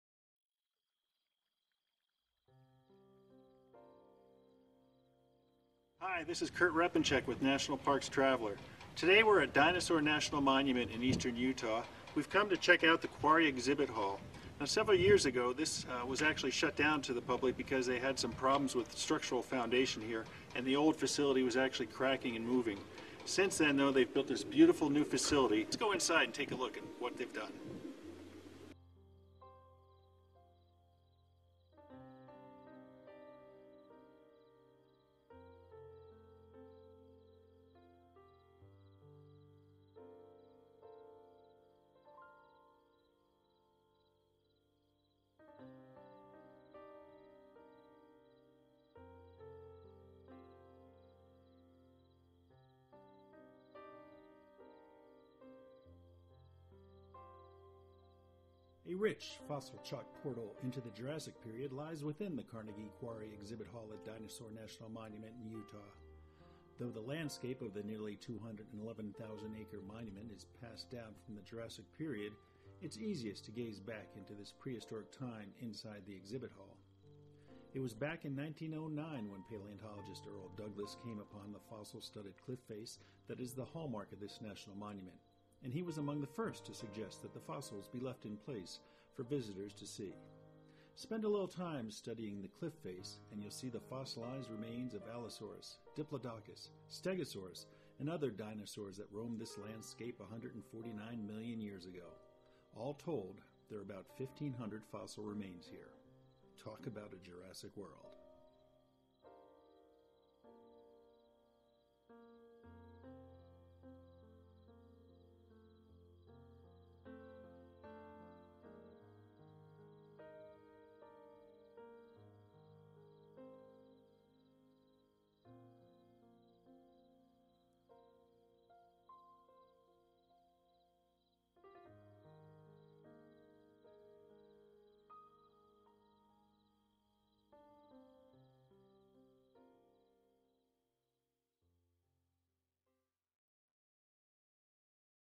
Exploring Dinosaur National Monument's Quarry Exhibit Hall
This short video takes you through the Quarry Exhibit Hall at Dinosaur National Monument, with a focal point of the fossil-embedded cliff face.